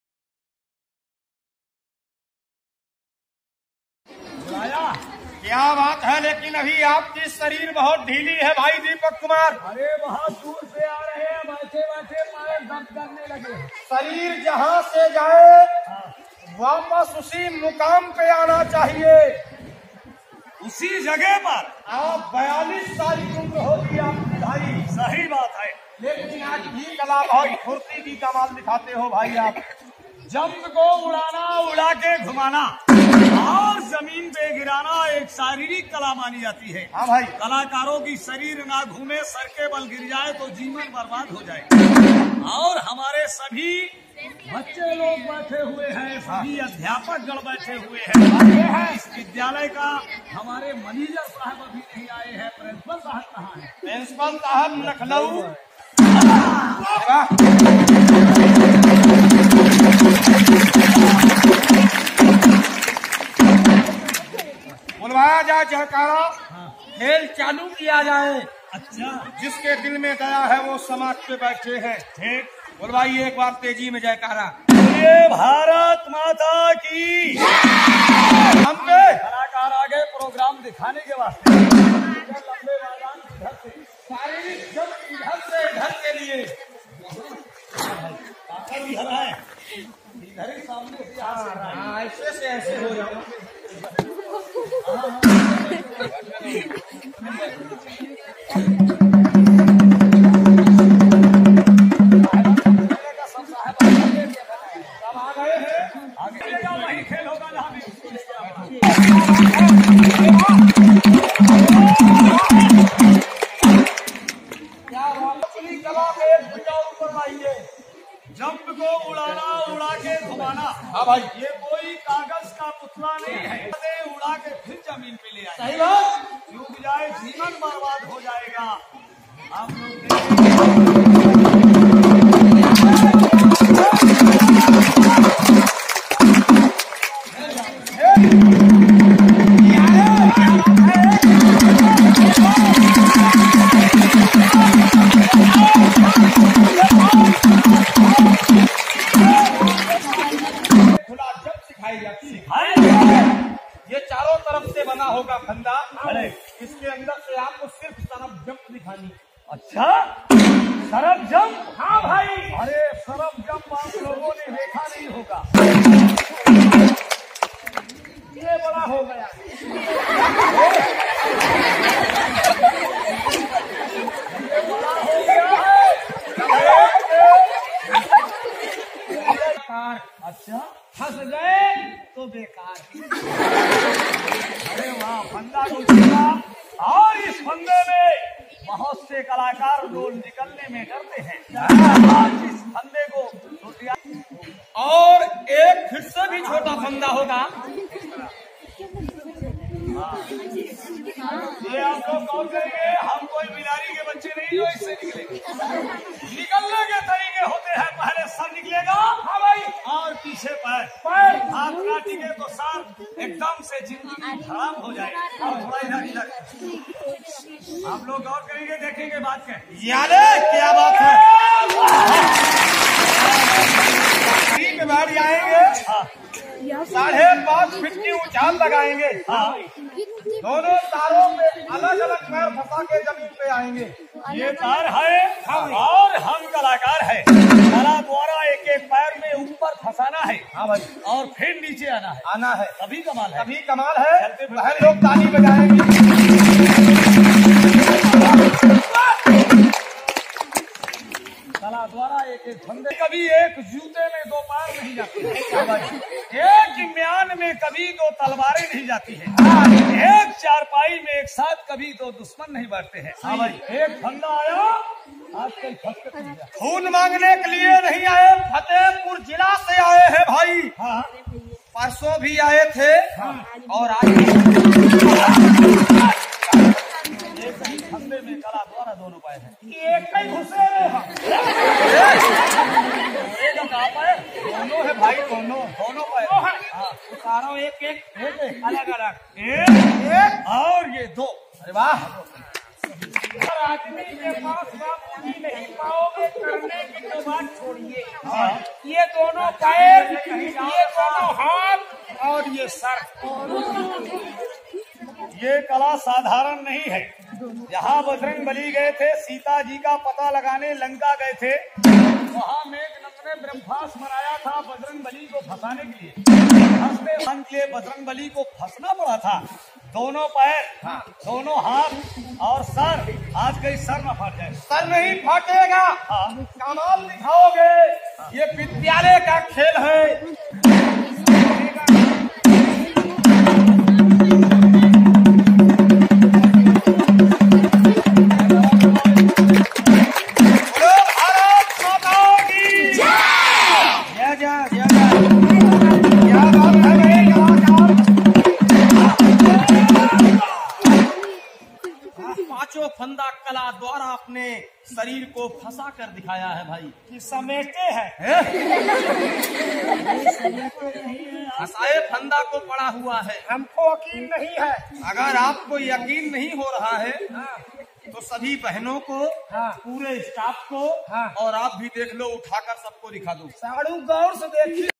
खेल तमाशा ऐसा करतब जो देखते रह जाओगे 10 साइकिल का वजन अपने पैरों पर आज जरूर देखो Game 1
diamondsharad खेल तमाशा ऐसा करतब जो देखते रह जाओगे 10 साइकिल का वजन अपने पैरों पर आज जरूर देखो Game 1 नट का खेल ऐसा खेल कभी ना देखा होगा कमाल की कलाबाजी